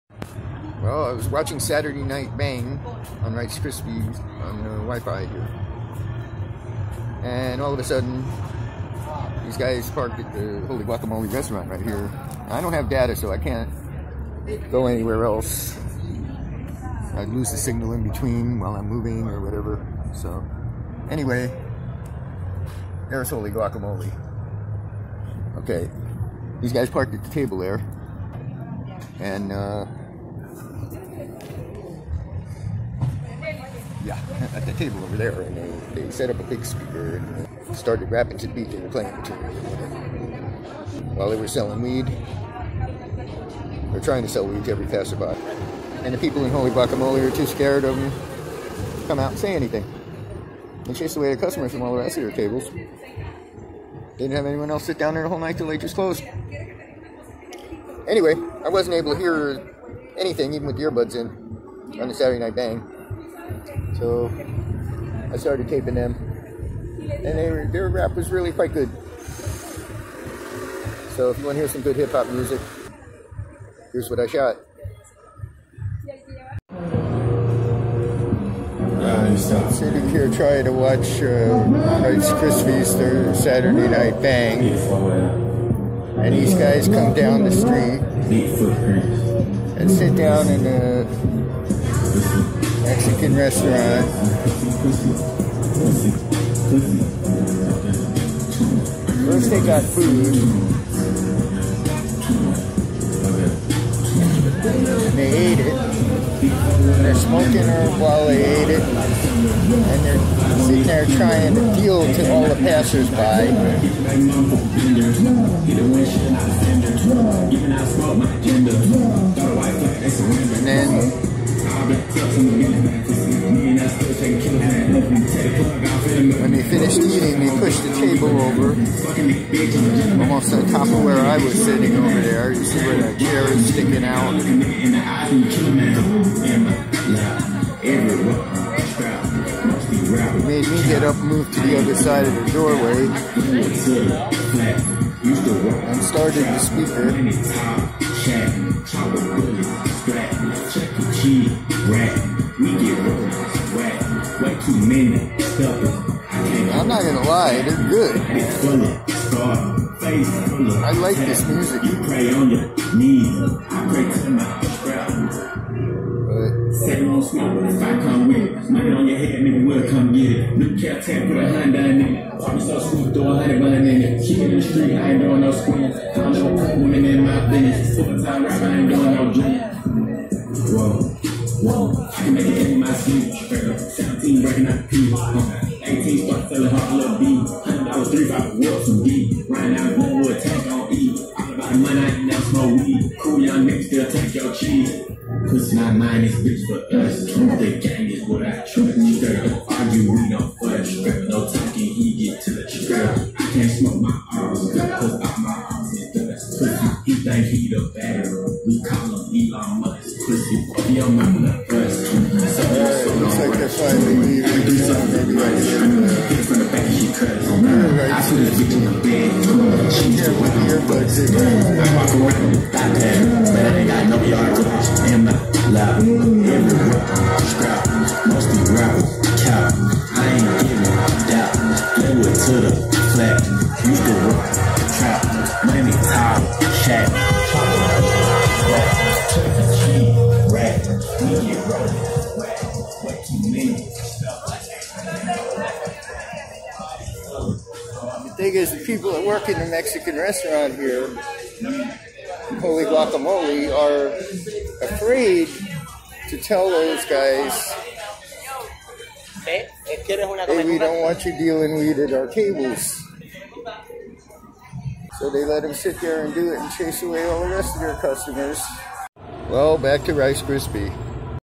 Rights Crispy Interrupted
2/13/21
1251 Washington Ave

I was watching RC's Saturday Night Bang when these guys started blasting hip-hop so I couldn't hear through my earbuds, so I started filming them.